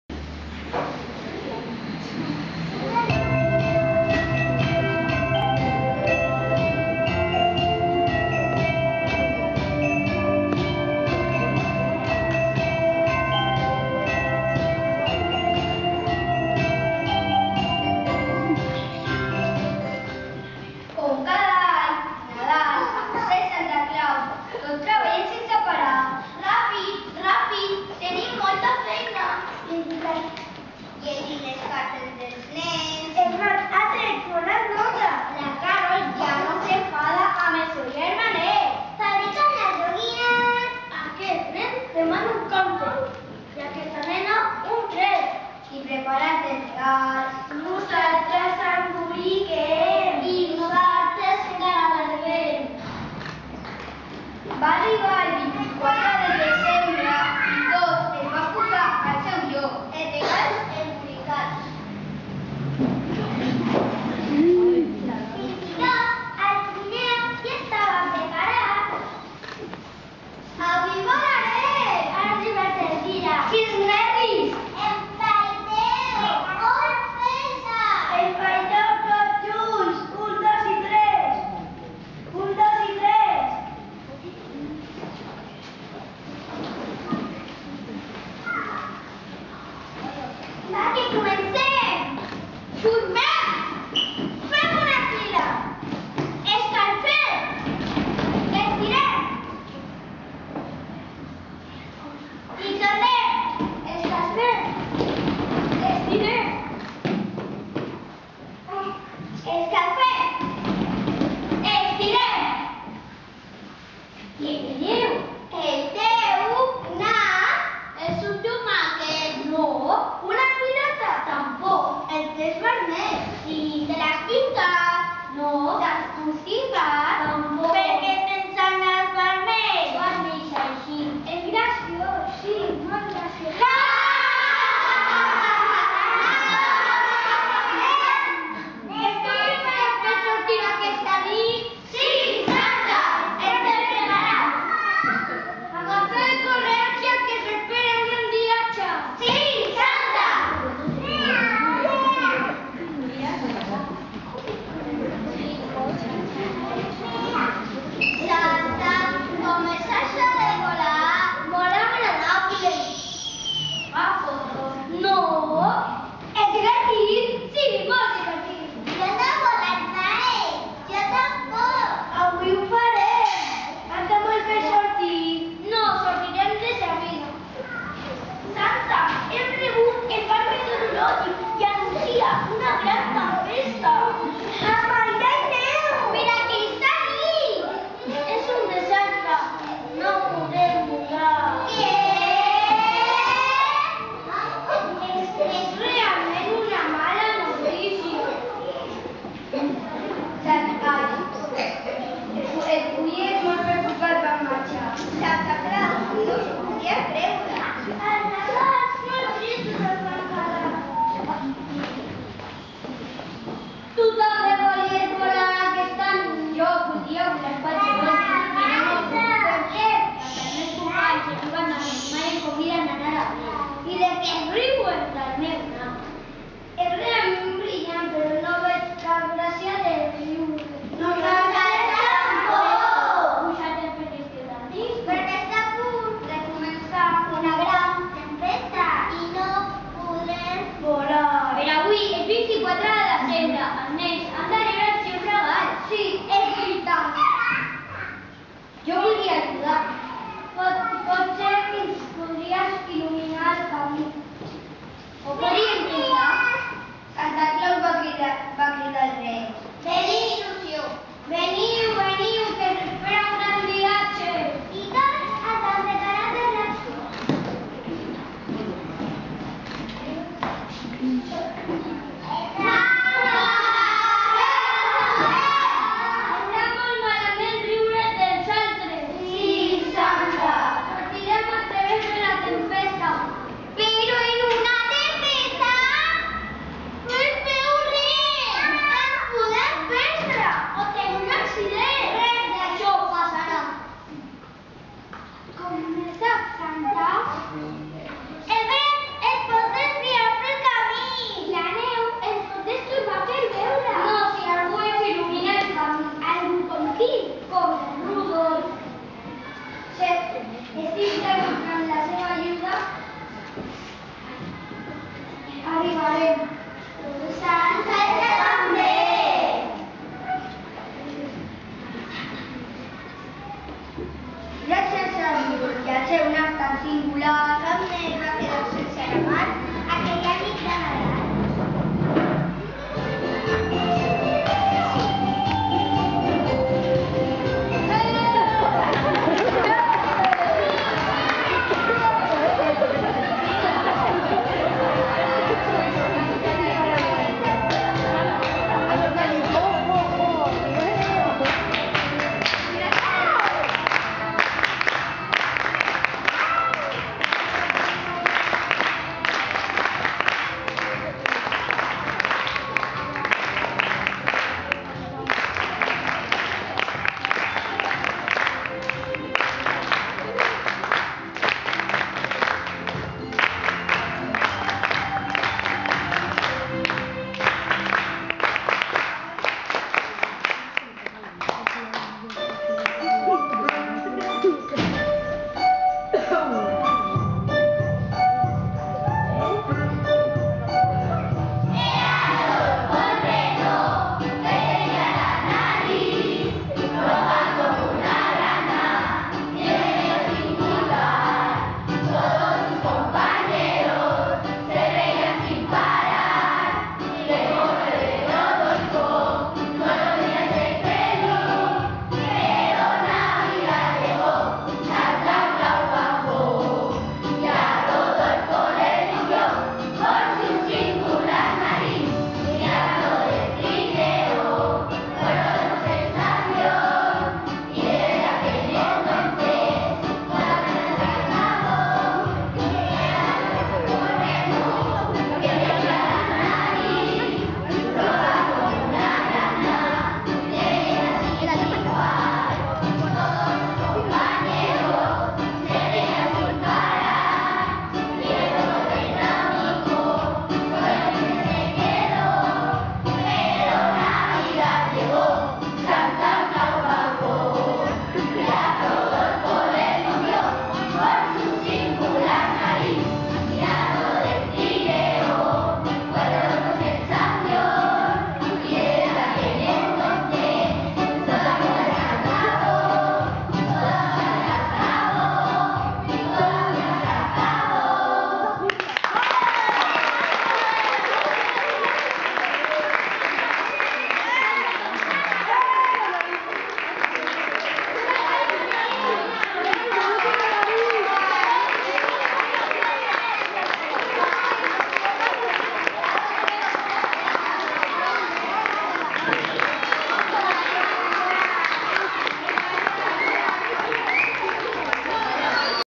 RUDOLPH EL REN
Els nens i nenes de 3r hem representat l'obra de teatre de Rudolph el ren dirigits per la Victòria Sojo.